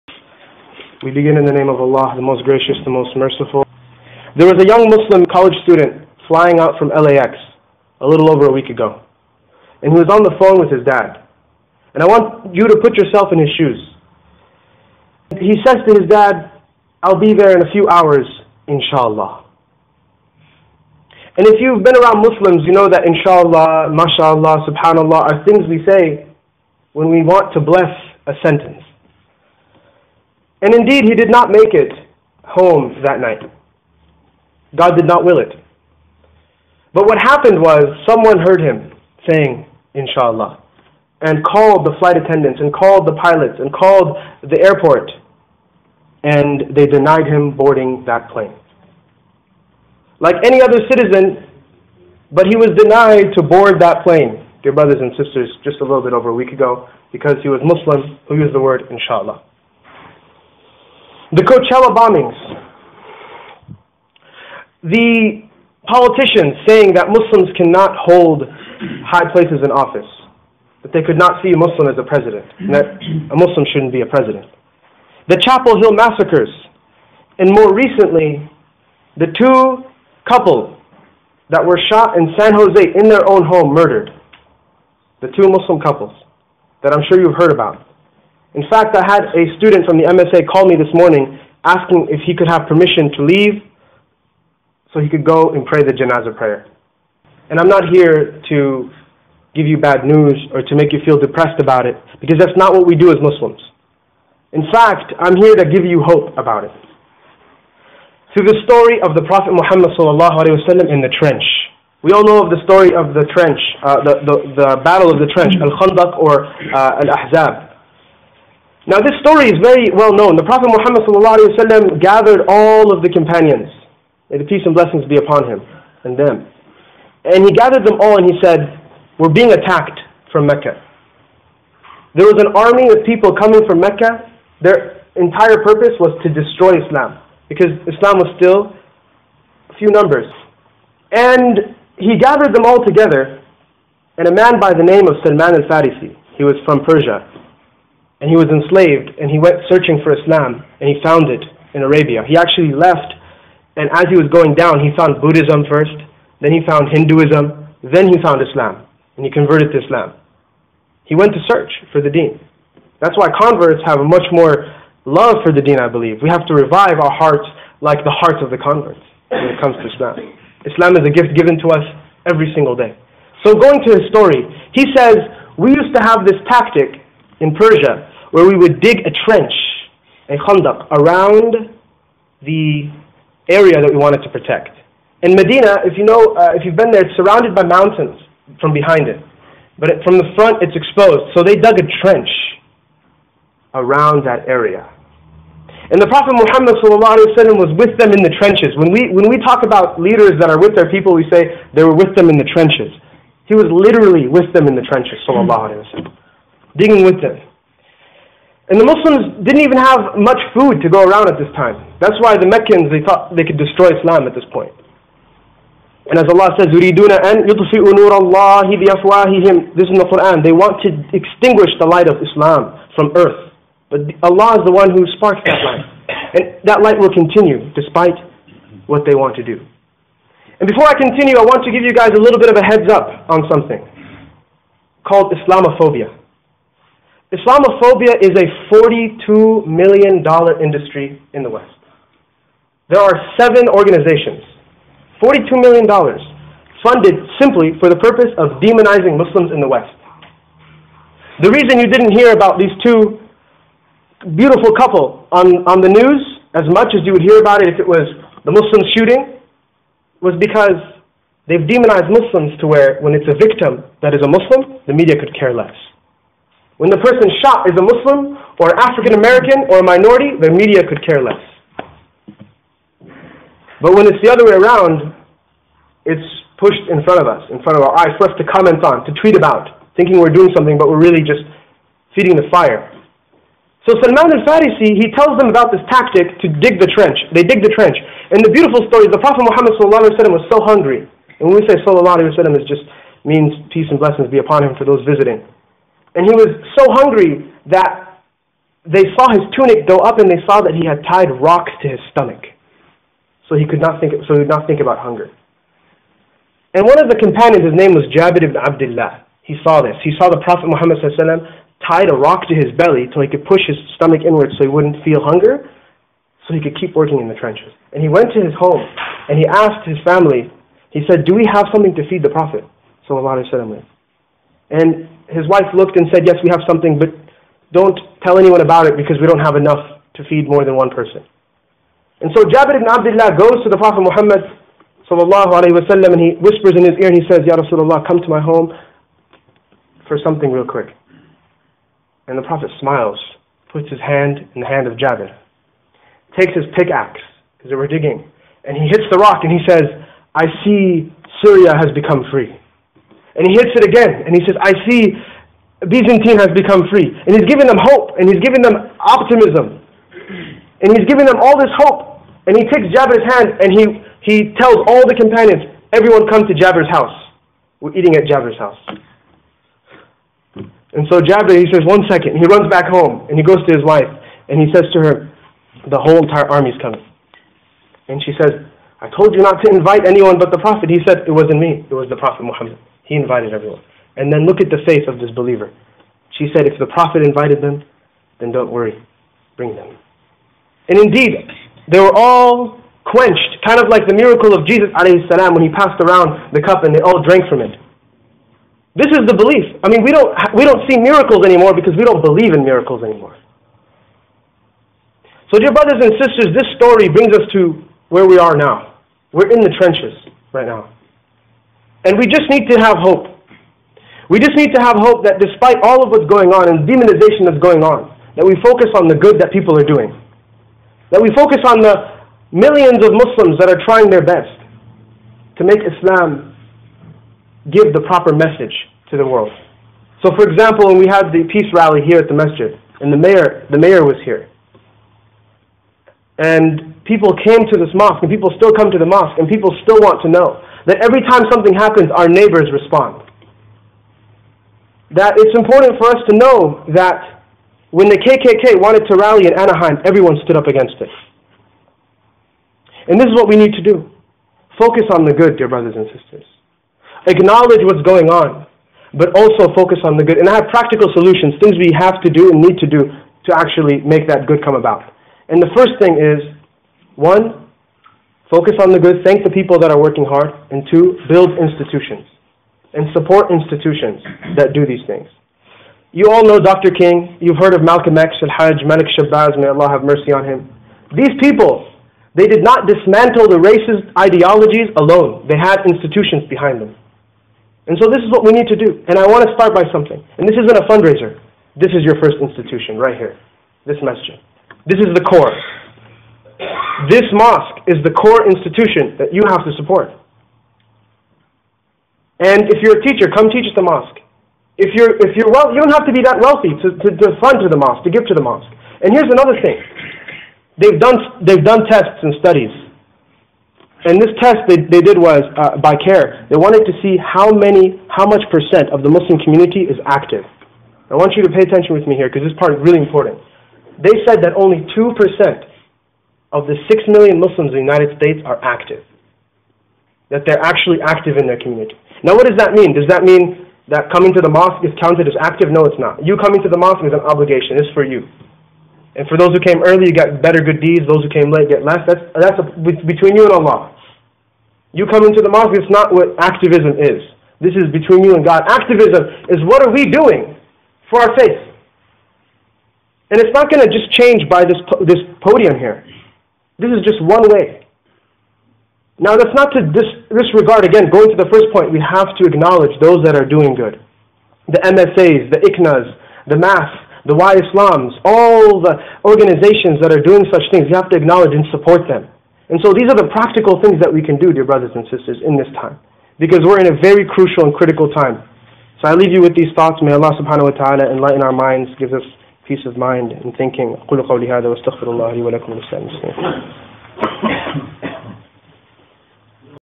Muslim Response to a Post-911 World
This video is about khutbah april 29th 2016